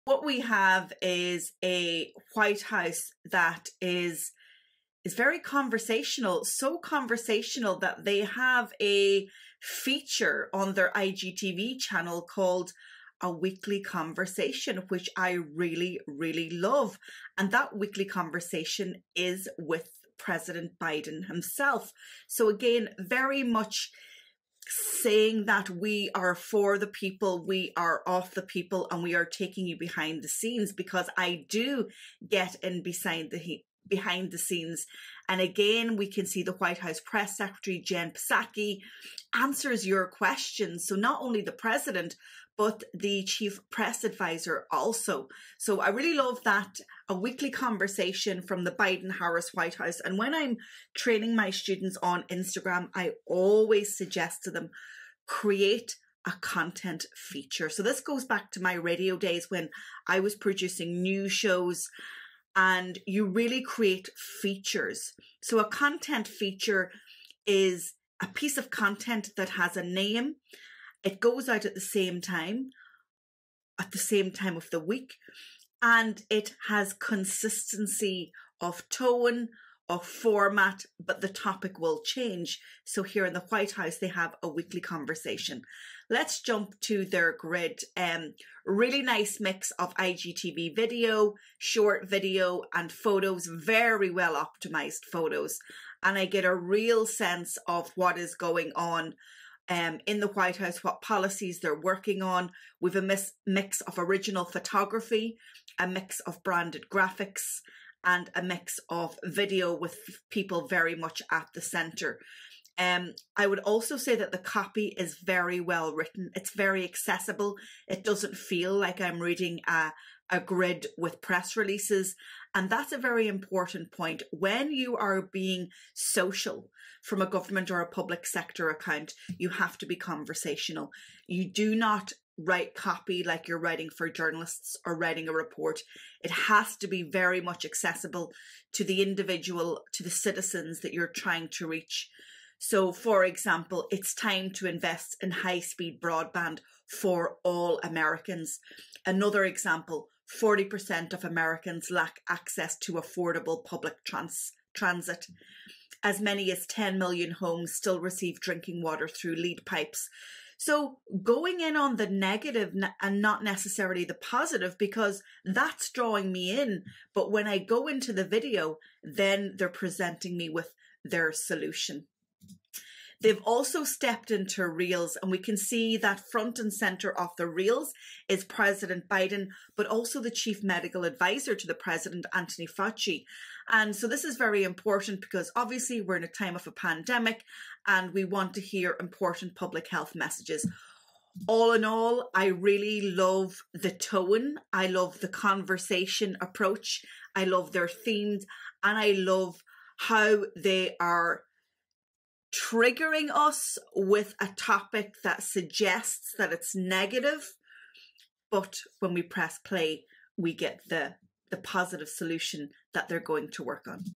What the White House is Doing Right on Instagram
What we have is a White House that is very conversational; so conversational that they have a feature on their IGTV channel called "A Weekly Conversation," which I really, really love. And that weekly conversation is with President Biden himself. So, again, very much saying that we are for the people, So, again, very much saying that we are for the people, we are of the people, and we are taking you behind the scenes, because I do get behind the scenes. And again, we can see the White House Press Secretary, Jen Psaki, answers your questions. So not only the president but the Chief Press Advisor, also. So, I really love that A Weekly Conversation from the Biden-Harris White House. And when I'm training my students on Instagram, I always suggest to them, create a content feature. So, this goes back to my radio days when I was producing new shows, and you really create features. So, a content feature is a piece of content that has a name; it goes out at the same time, at the same time of the week, and it has consistency of tone, of format, and it has consistency of tone, of format, but the topic will change. So, here at the White House, they have A Weekly Conversation. Let's jump to their grid. And really nice mix of IGTV video, short video, and photos, very well-optimized photos. And I get a real sense of what is going on in the White House; what policies they're working on with a mix of original photography, a mix of branded graphics, and a mix of video with people very much at the centre. I would also say that the copy is very well-written. It's very accessible. It doesn't feel like I'm reading a grid with press releases. And that's a very important point. When you are being social from a government or a public sector or kind, you have to be conversational. You do not write copy like you're writing for journalists or writing a report. It has to be very much accessible to the individual, to the citizens that you're trying to reach. So, for example, "It's time to invest in high-speed broadband for all Americans." Another example, "40% of Americans lack access to affordable public transit." "As many as 10 million homes still receive drinking water through lead pipes." So, going in on the negative and not necessarily the positive because that's drawing me in. But when I go into the video, then they're presenting me with their solution. They've also stepped into Reels. And we can see that front and centre of the Reels is President Biden, but also the Chief Medical Advisor to the President, Anthony Fauci. And so this is very important because obviously we're in a time of a pandemic and we want to hear important public health messages. All in all, I really love the tone, I love the conversation approach, I love their themes, and I love how they are triggering us and I love how they are triggering us with a topic that suggests that it's negative but when we press Play, we get the positive solution that they're going to work on.